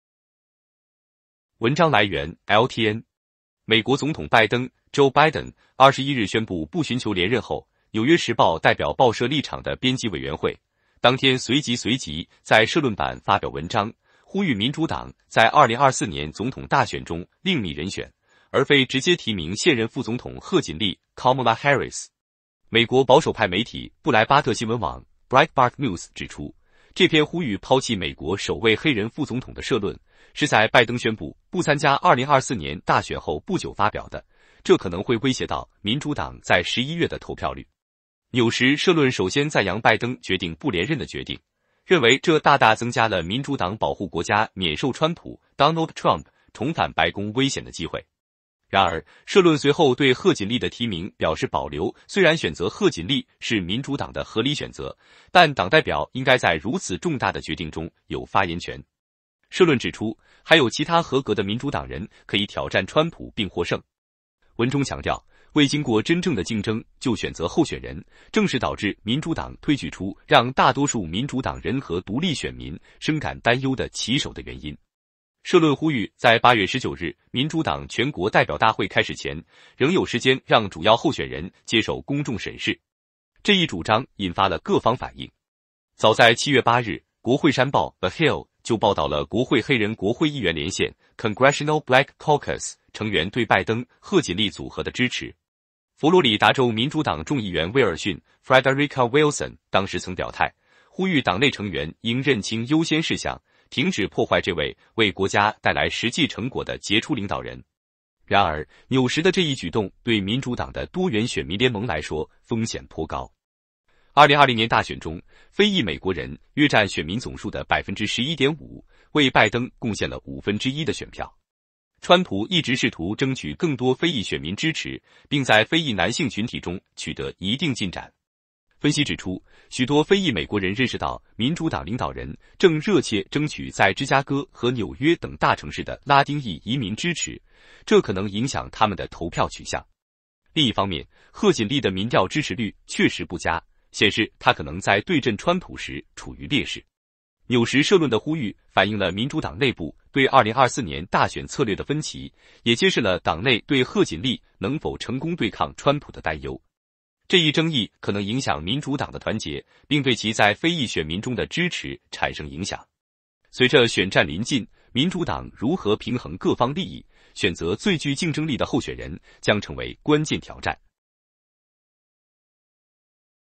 纽约时报不挺贺锦丽！ 力促民主党另觅人选
文章来源: LTN
美国总统拜登（Joe Biden）21日宣布不寻求连任后，《纽约时报》代表报社立场的编辑委员会，当天随即随即在社论版发表文章，呼吁民主党在2024年总统大选中另觅人选，而非直接提名现任副总统贺锦丽（Kamala Harris）。美